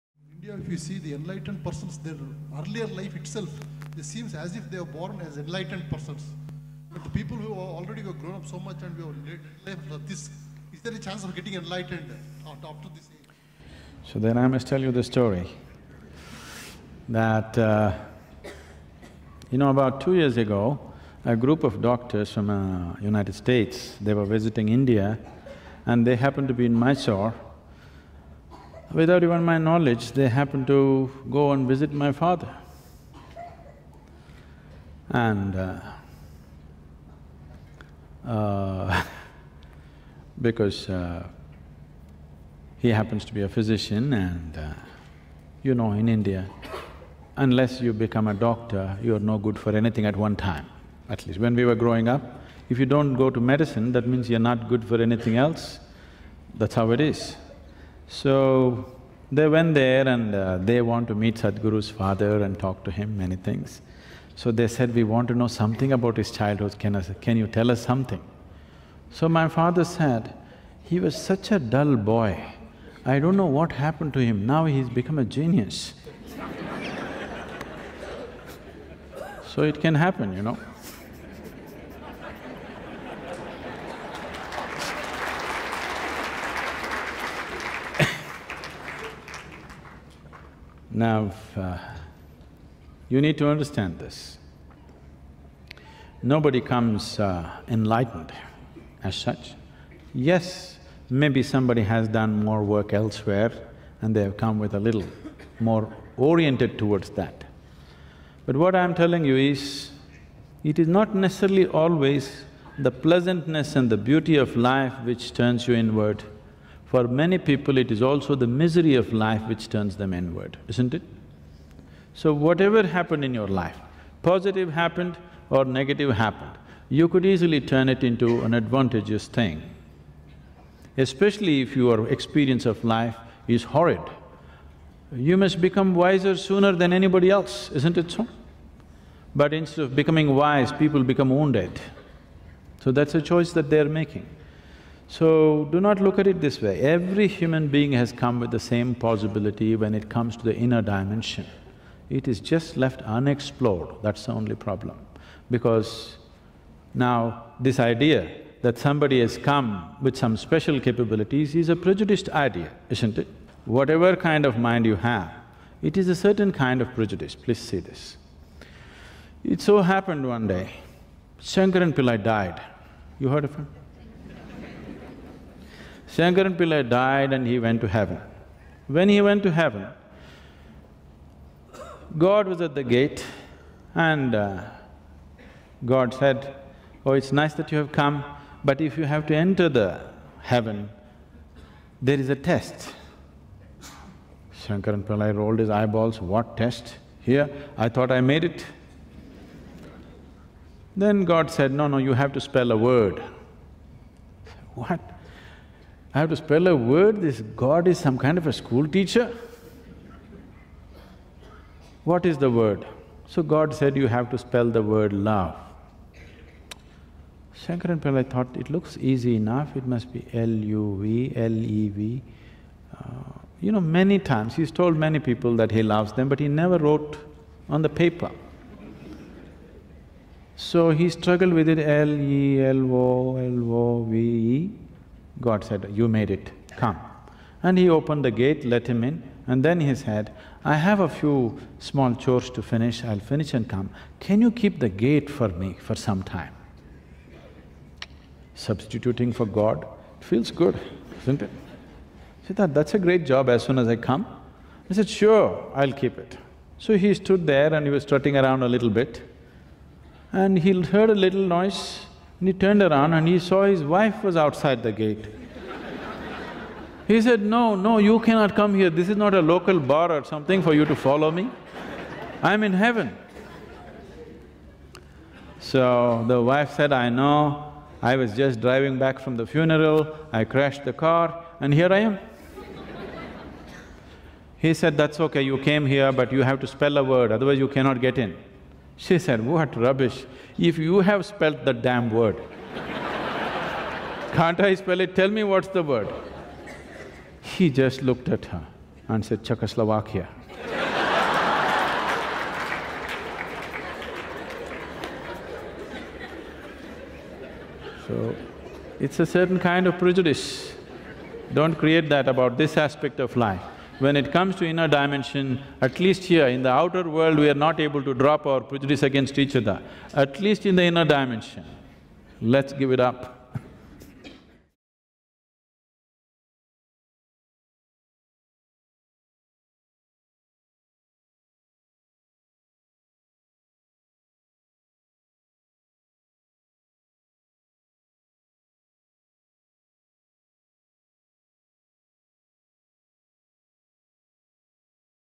Is there any chance of me getting enlightened ?Sadhguru in Bangalore, July 2011
Music Credits -
Video Clip Credits -